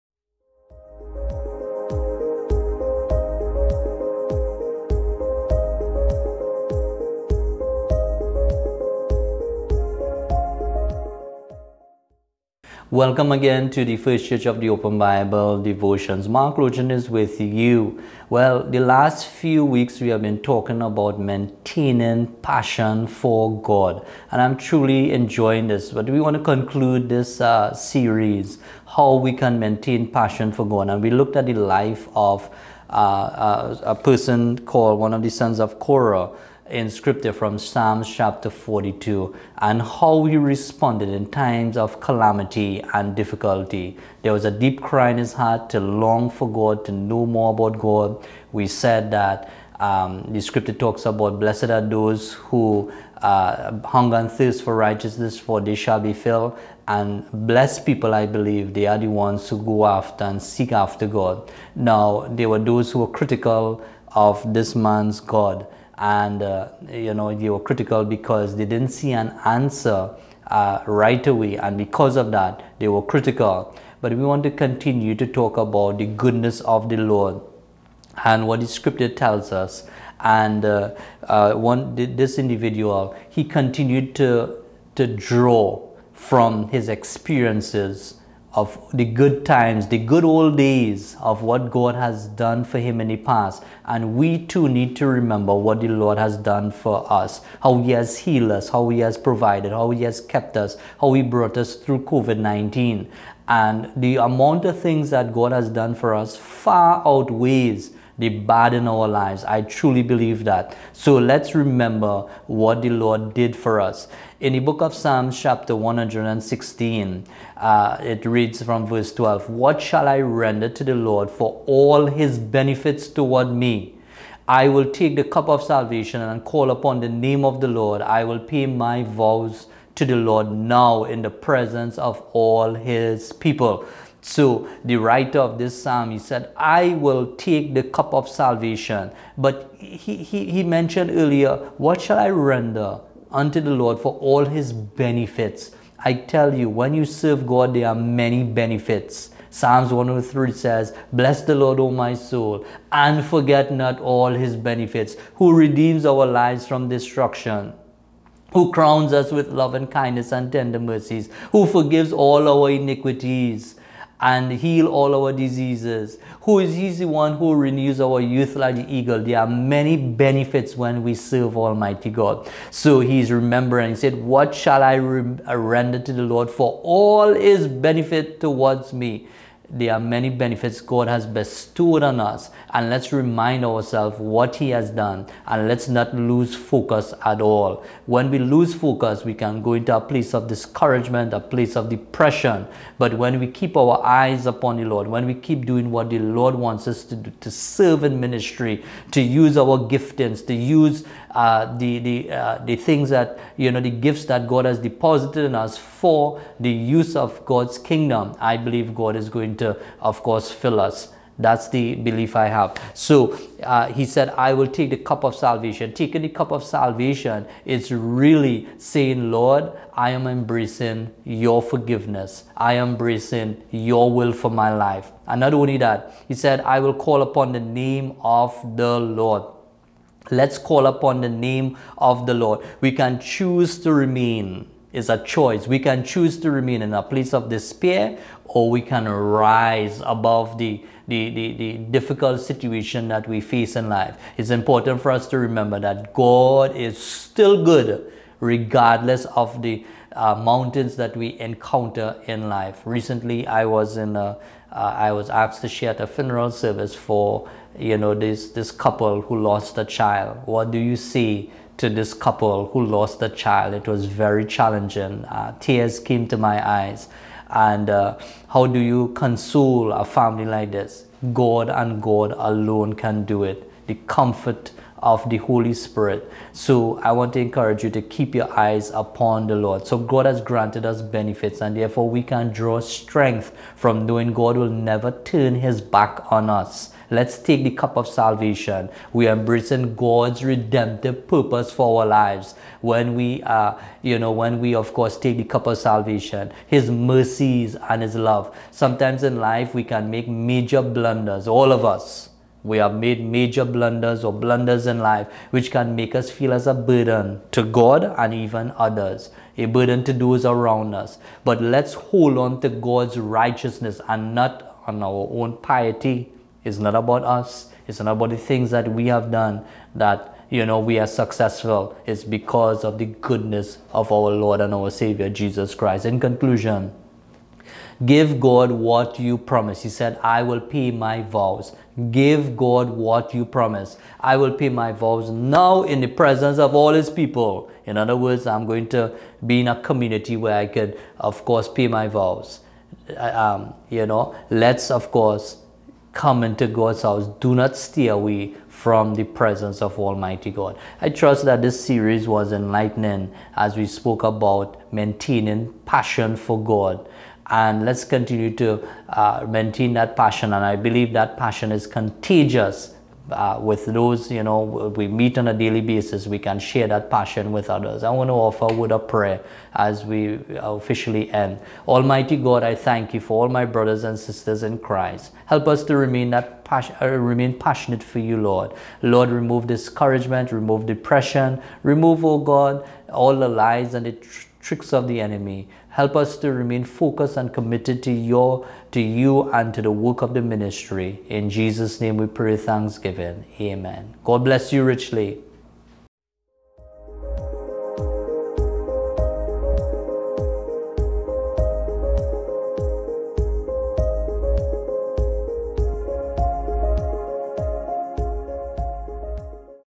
Maintaining Passion for God (Part 8) with Pas. Mark Lochan - 10th October, 2024
God's Promises Series: Maintaining Passion for God (Part 8) with Pas. Mark Lochan - 10th October, 2024